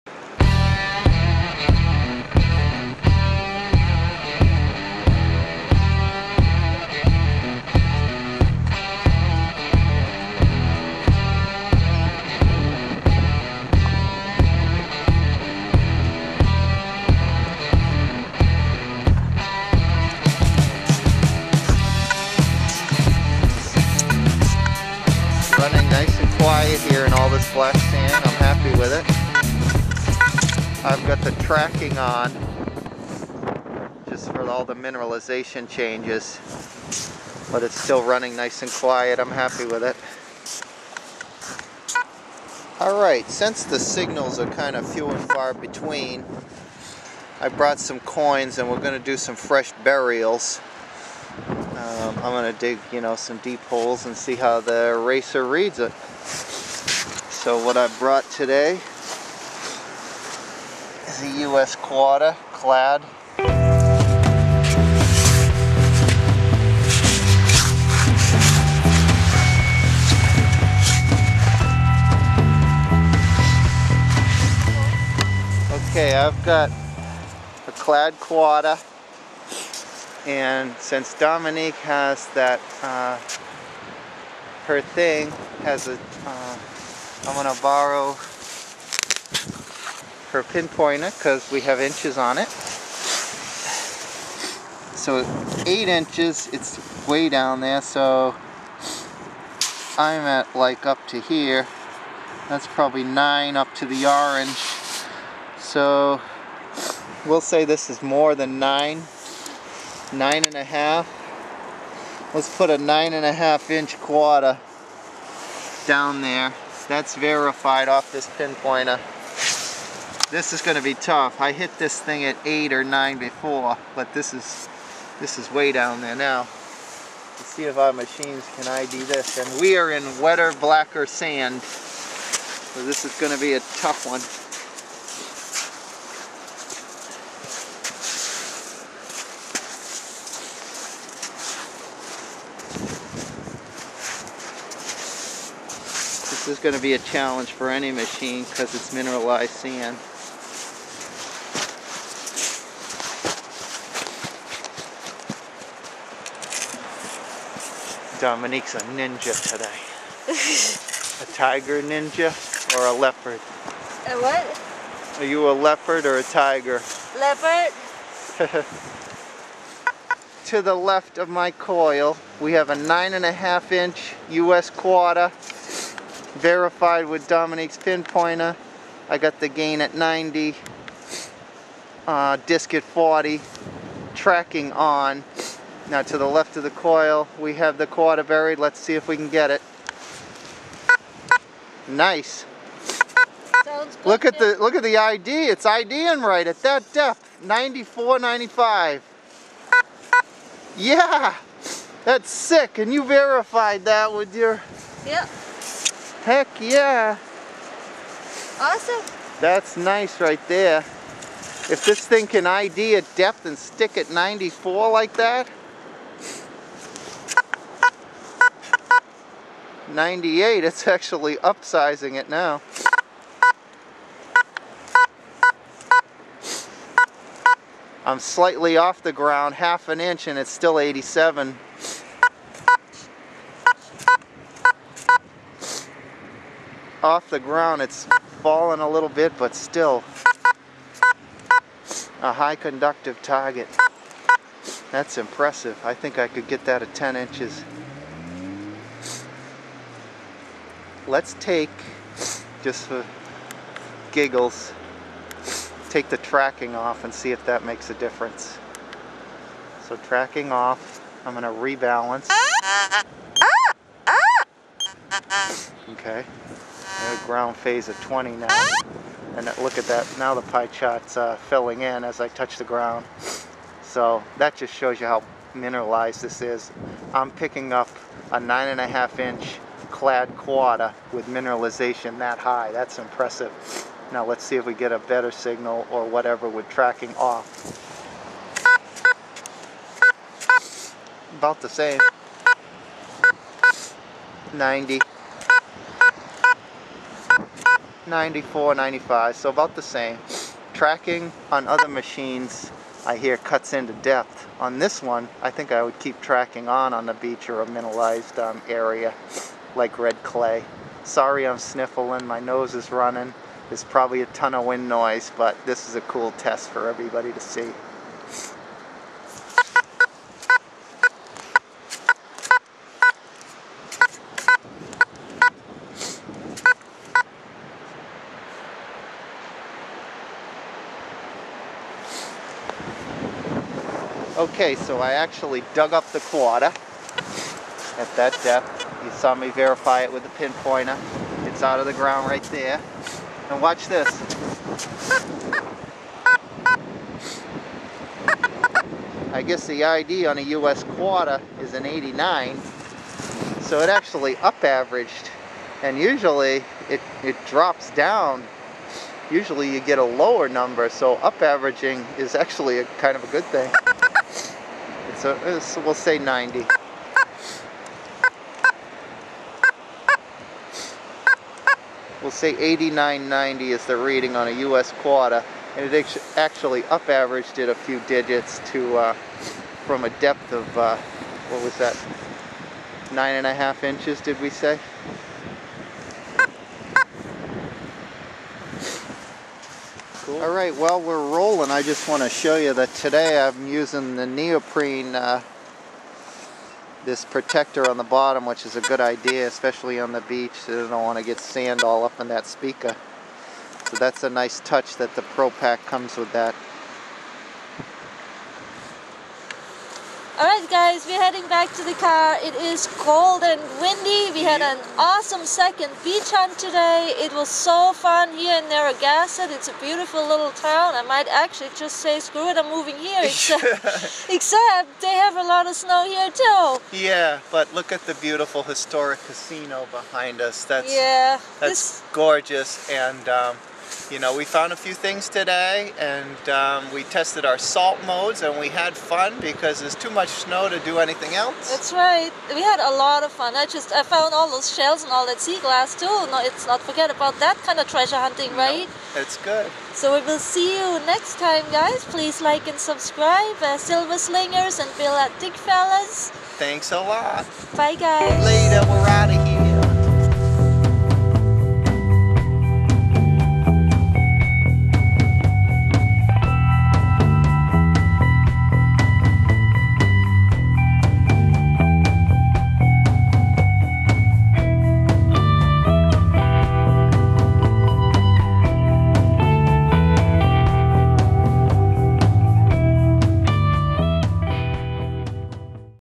Makro Racer Field Test on wet sand 10" coin metal detecting, racer 2 detector
Watch the new Makro Racer hit a 10" quarter on a field test by Bill Ladd & Dominique Ivy (The Silverslingers) on historic Narragansett Beach in RI.